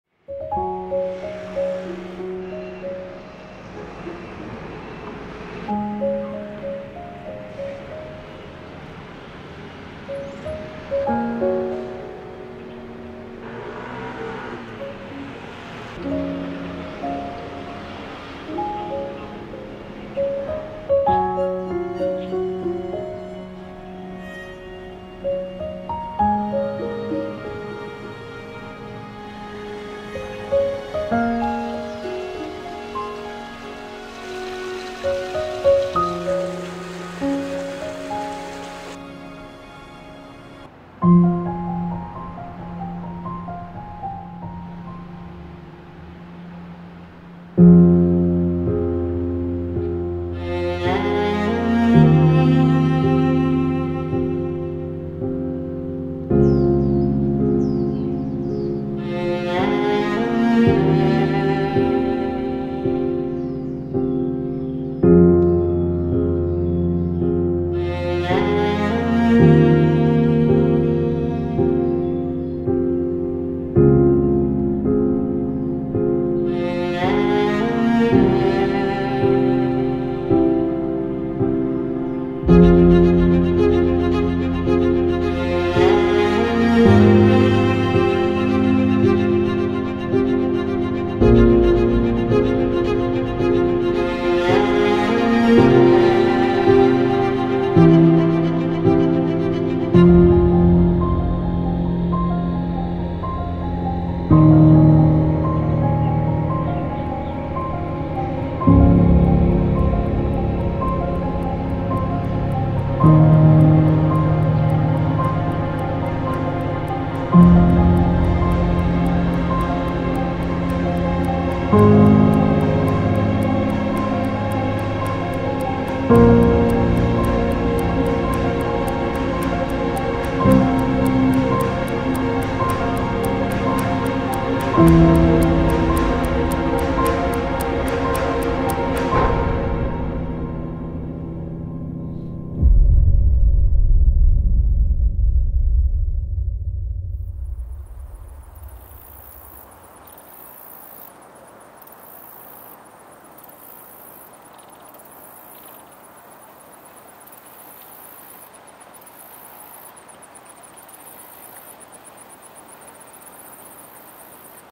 Uncovering Taipei's Confucius Temple: A Journey Through History
September 28th marks Confucius's birthday, also celebrated as Teacher's Day in China and Taiwan. Confucius is a paragon of teachers in Chinese history, symbolizing the philosophy of educating everyone without distinction. This video takes you on a journey through Taipei's Confucius Temple, exploring its rich history and cultural significance.